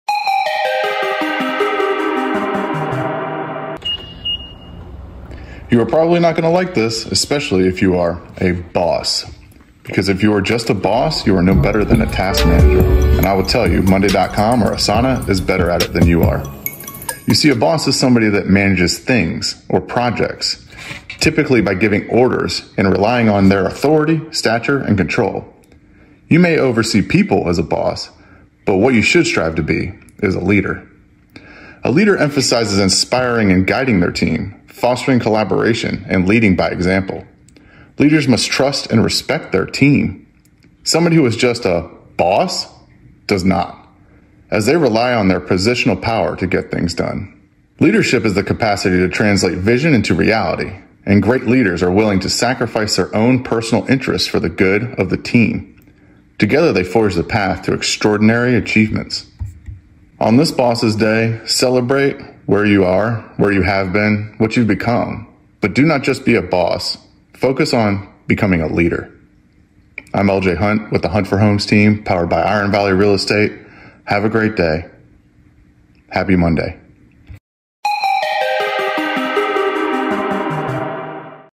Success isn't measured by transactions it's measured by the lives you positively impact
"Success in real estate isn't measured by transactions; it's measured by the lives you positively impact. Cheers to a boss who understands this well. Happy Boss Day!"

If you want to chat about your next move.